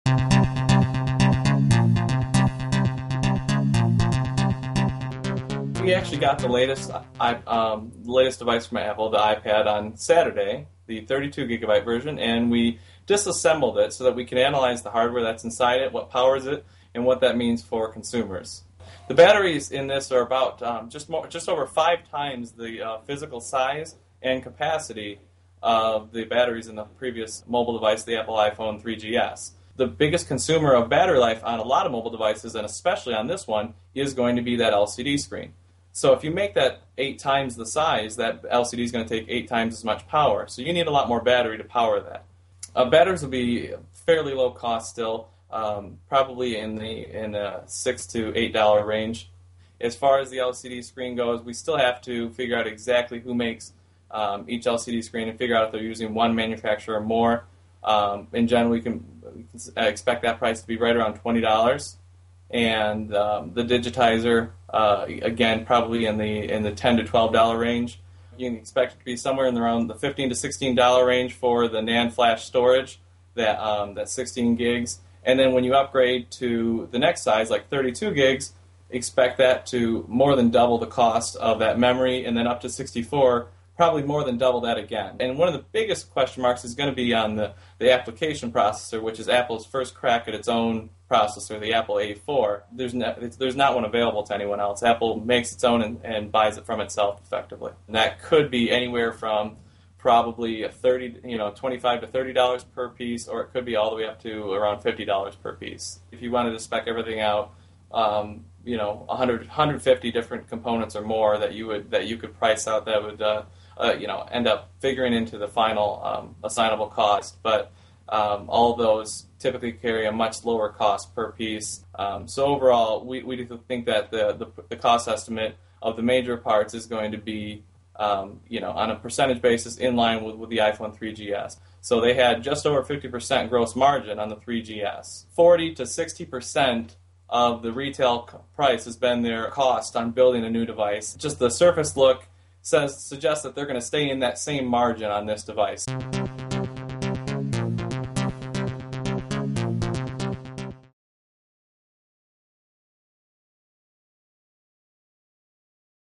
What it costs to make an iPad
A look at the parts inside the device and how they add up.